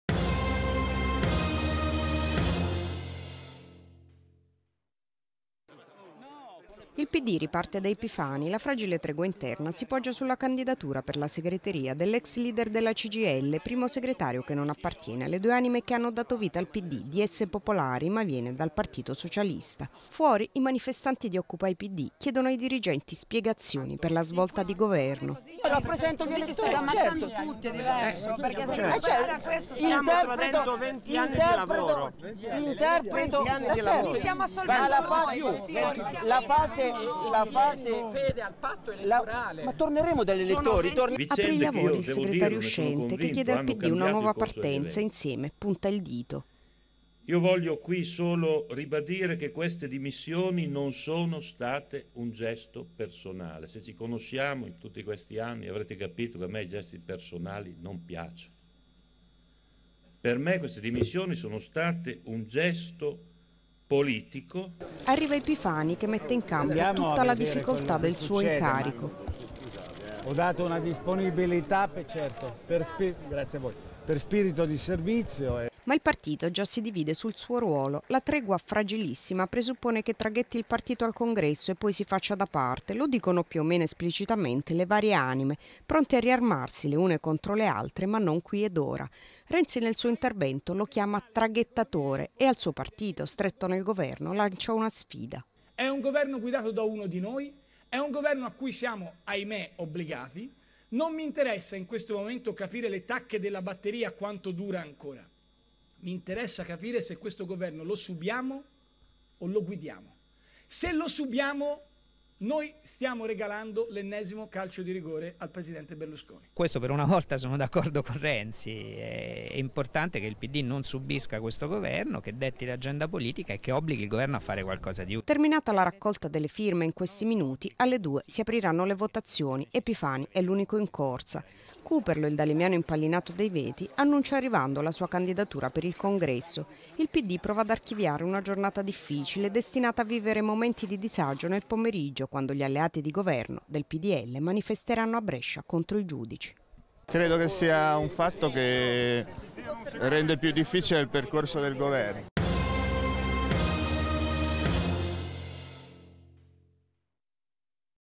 Il Pd si affida a Epifani, segreatrio fino al congresso
Salvo colpi di scena l'assemblea Pd dovrà eleggere Guglielmo Epifani segretario traghettatore. "Ho accettato per spirito di servizio", ha detto l'ex leader Cgil. Renzi invoca un cambio di marcia tra i democratici: non regaliamo il governo a Berlusconi, le priorità a Letta dobbiamo darle noi. Servizio di Marinella Aresta